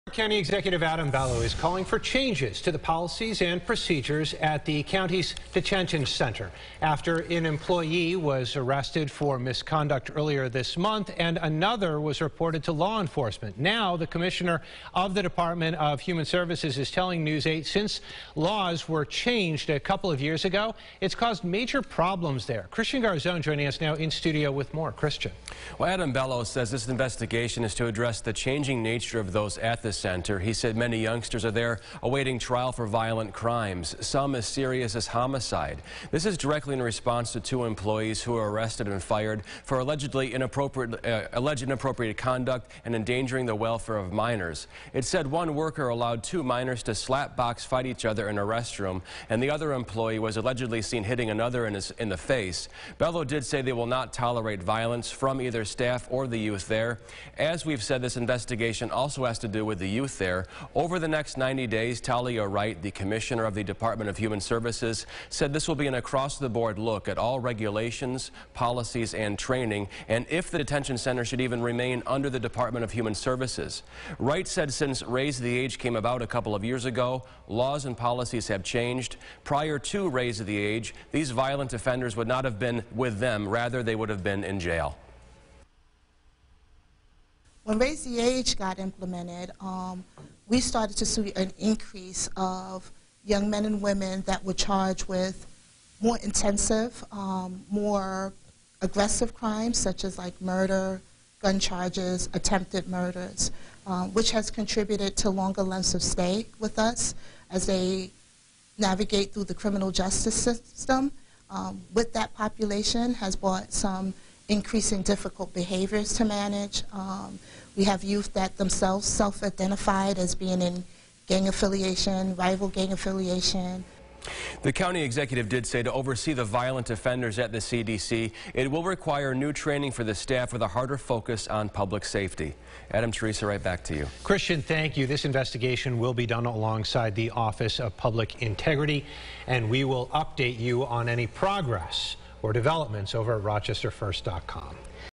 After 'Raise the Age' law, Monroe County calls for changes at Children's Detention Center
Monroe County Executive Adam Bello released a statement Wednesday calling on the Monroe County Office of Public Integrity (OPI) to review and recommend changes to the policies and procedures at the Monroe County Children's Detention Center.